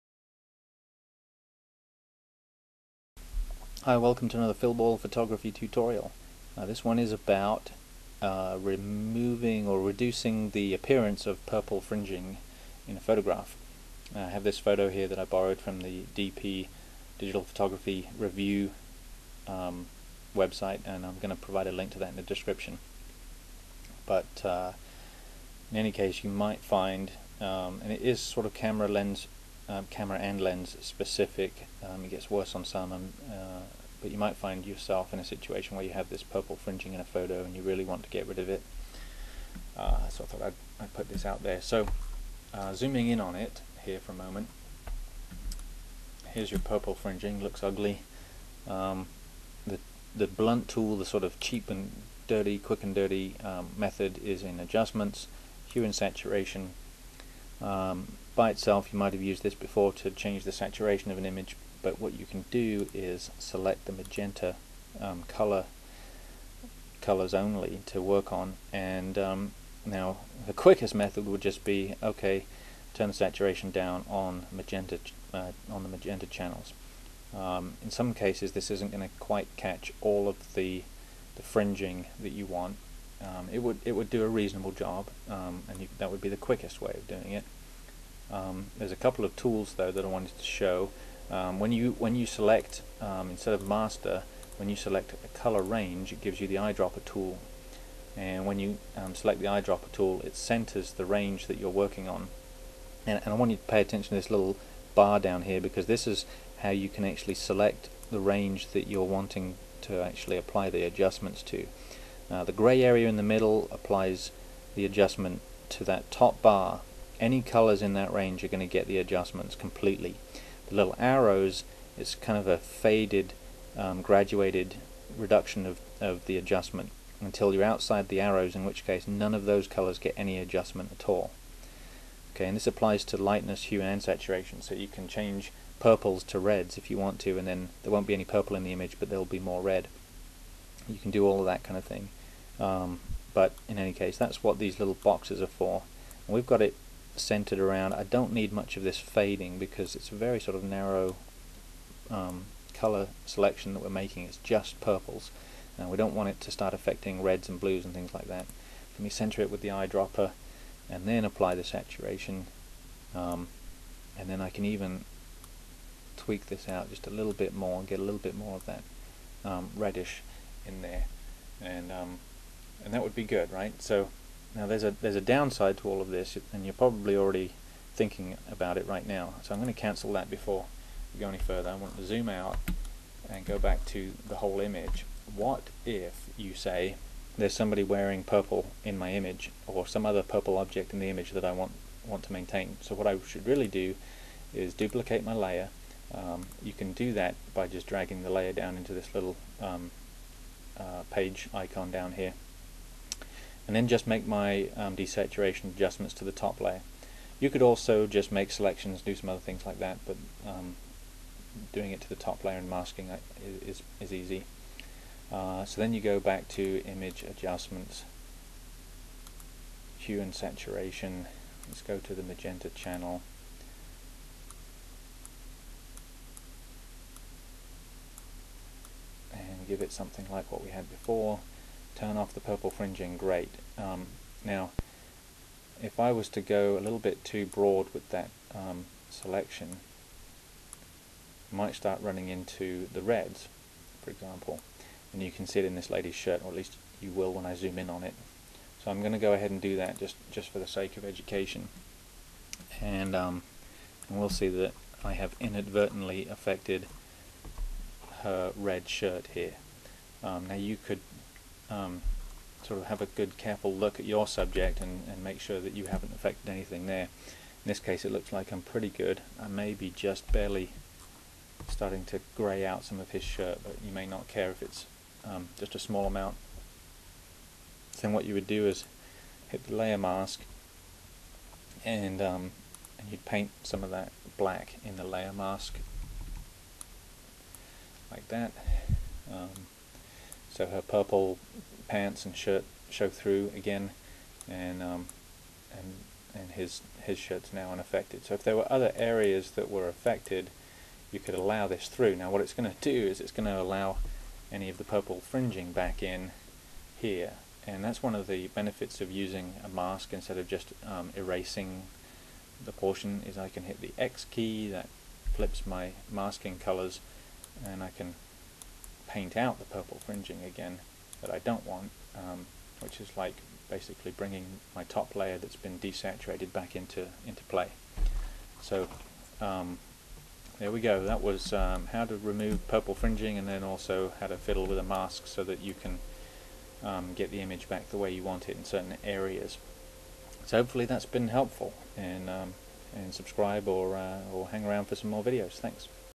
how to remove purple fringing from a photo in photoshop
Quick 5 minute tutorial covering the process of selectively de-saturating the offending colors, and using a layer mask to protect areas of the image that might be inadvertently affected.

for the science behind purple fringing (and chromatic aberration) check out

That's also where I got the image from to do this tutorial so you can try it yourself if you want.